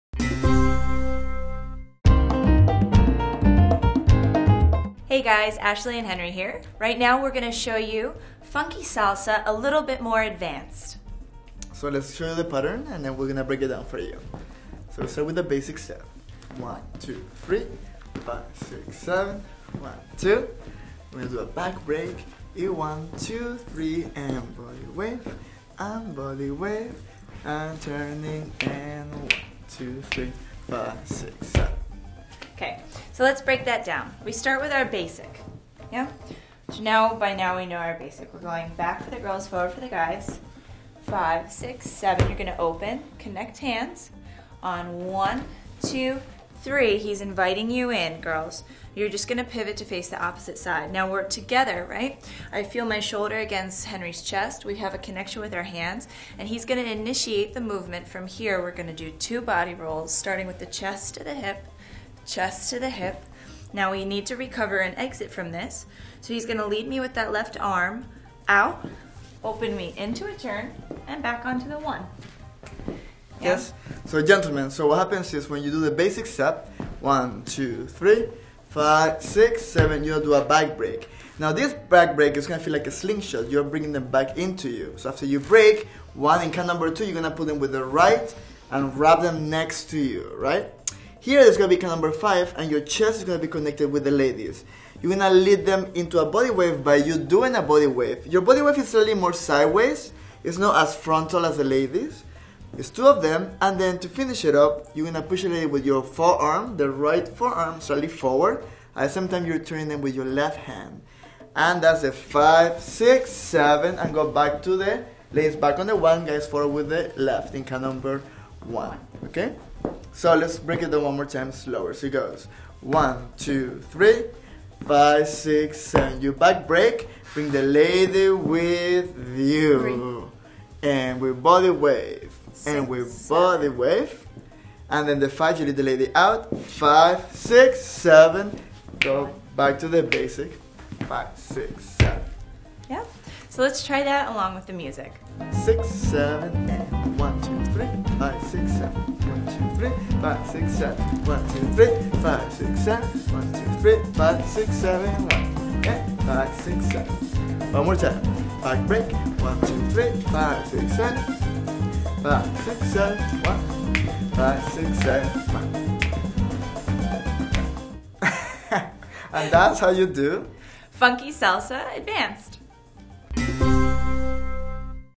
How to Do Advanced Funky Salsa Moves | Latin Dance
Like these Ballroom Dance Lessons !!!

Ashley: Hey guys, Ashley and Henry here. Right now we're gonna show you Funky Salsa, a little bit more advanced.

Henry: So let's show you the pattern, and then we're going to break it down for you. So start with the basic step. One, two, three, five, six, seven, one, two. We're gonna do a back break. And one, two, three and roll away, and roll away, and turning, and one, two, three, five, six, seven.

Ashley: OK. So let's break that down. We start with our basic. So by now we know our basic, going back for the girls, forward for the guys. Five, six, seven, you're going to open, connect hands. On one, two, three, he's inviting you in, girls. You're just going to pivot to face the opposite side.

Now, we're together, right? I feel my shoulder against Henry's chest. We have a connection with our hands, and he's going to initiate the movement from here. We're going to do two body rolls, starting with the chest to the hip, chest to the hip. Now, we need to recover and exit from this. So he's going to lead me with that left arm, out, open me into a turn, and back onto the one.

Henry: Yes? So gentlemen, what happens is when you do the basic step, one, two, three, five, six, seven, you'll do a back break. Now this back break is gonna feel like a slingshot, you're bringing them back into you. So after you break, one, and cut number two, you're going to pull them with the right and grab them next to you, right? Here is going to be count number five, and your chest is going to be connected with the lady's. You're gonna lead them into a body wave. By you doing a body wave, your body wave is really more sideways, it's not as frontal as the ladies. It's two of them, and then to finish it up, you're gonna push away with your forearm, the right forearm. So lean forward, at the same time you're turning them with your left hand. And that's the five, six, seven, and go back to the, ladies back on the one. Guys forward with the left, and count number one. OK?

So let's break it down one more time, slower. So it goes, one, two, three, five, six, seven. You back break, bring the lady with you. And we body wave, and we body wave. And then the five, you lead the lady out, five, six, seven. Go back to the basic, five, six, seven.

Ashley: Yeah? So let's try that along with the music.

Henry: Six, seven and, one, two, three, five, six, seven, one, two, three, five, six, seven, one, two, three, five, six, seven, one, two, three, five, six, seven, one, two, three, five, six, seven. One more time. Back break. One, two, three, five, six, seven, five, six, seven, one, five, six, seven ... And that's how you do ...

Ashley: Funky Salsa Advanced!